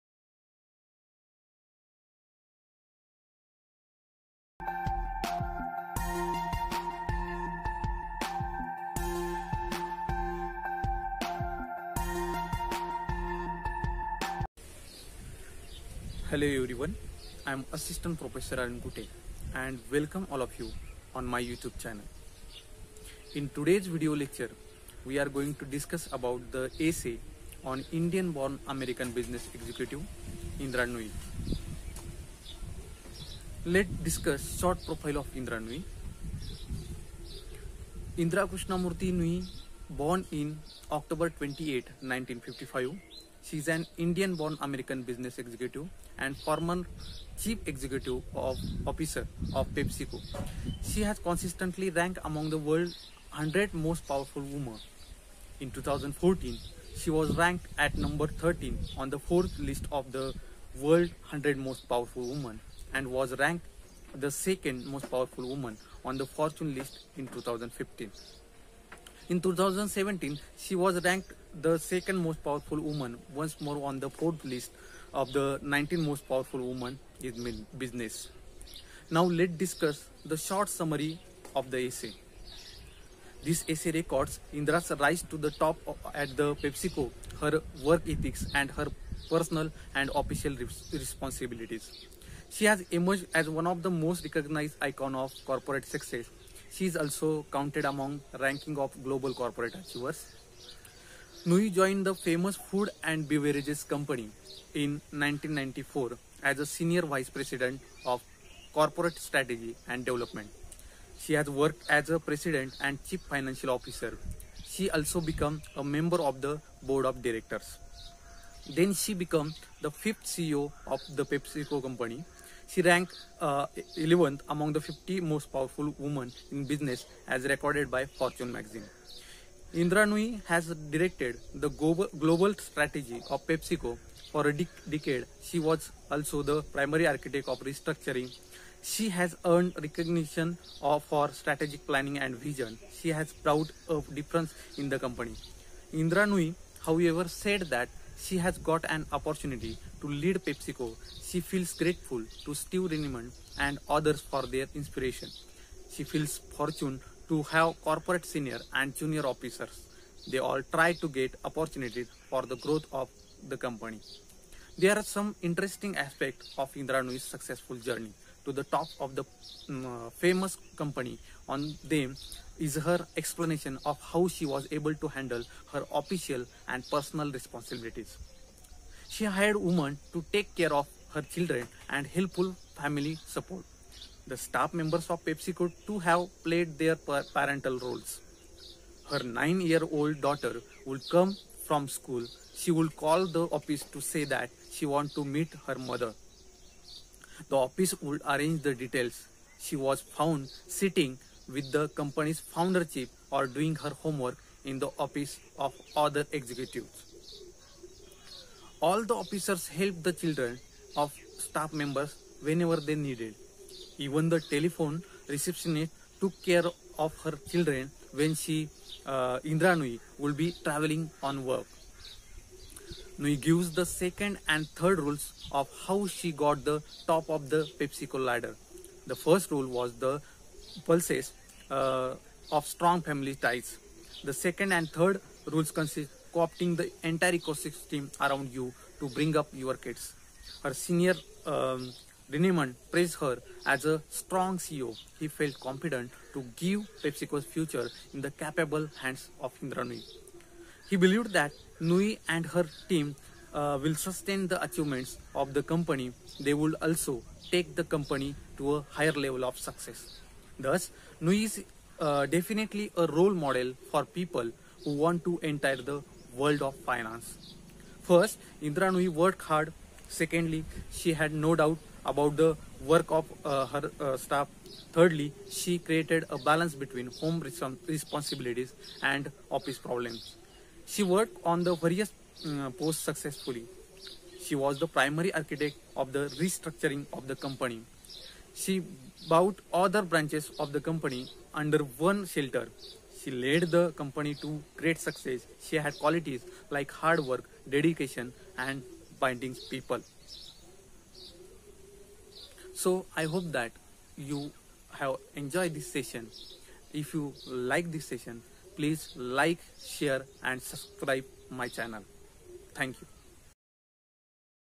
Indra Nooyi: A Corporate Giant.....an explanation by Mr. Arvind Kute
F.Y.B.Com (Rev-2019) Semester-II. Unit-1

SPPU, Pune Syllabus

Thanks

Prof. Dr. Ashish and, Varsha Publication

Wikipedia and internet sources